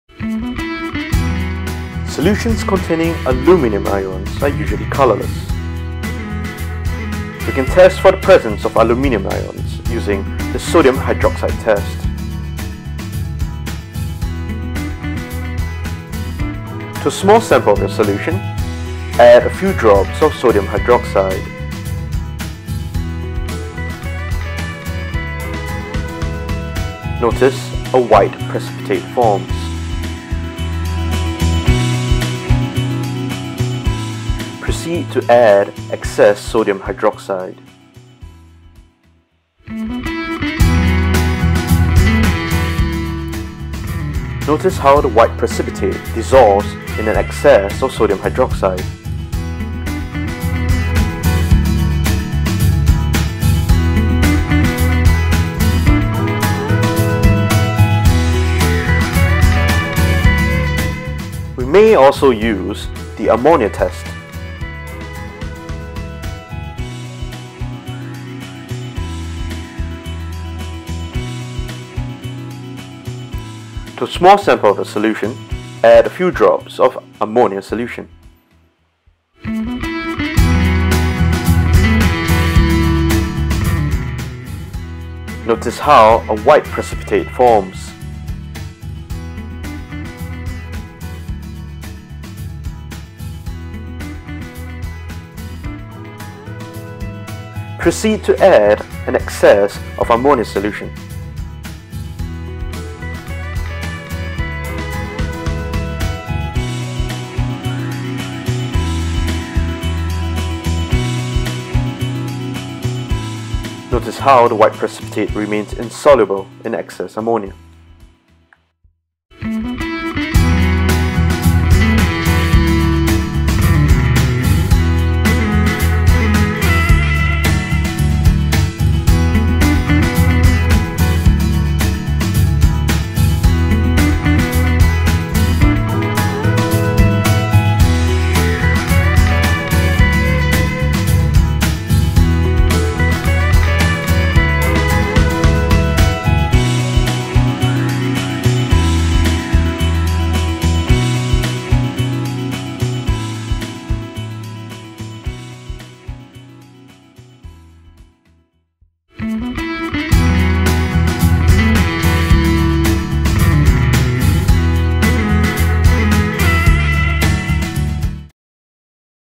Cation Test: Aluminium Ions
Solutions containing aluminium ions are typically colourless in colour.

To test for the presence of aluminium ions in solution, you may use either the sodium hydroxide test or the ammonia test.

1) Sodium Hydroxide Test - To a sample of your solution, add a few drops of sodium hydroxide solution. A white precipitate forms. Upon addition of excess sodium hydroxide, the white precipitate dissolves.

2) Ammonia Test - To a sample of your solution, add a few drops of ammonia solution. A white precipitate forms. Upon addition of excess ammonia solution, the white precipitate remains insoluble.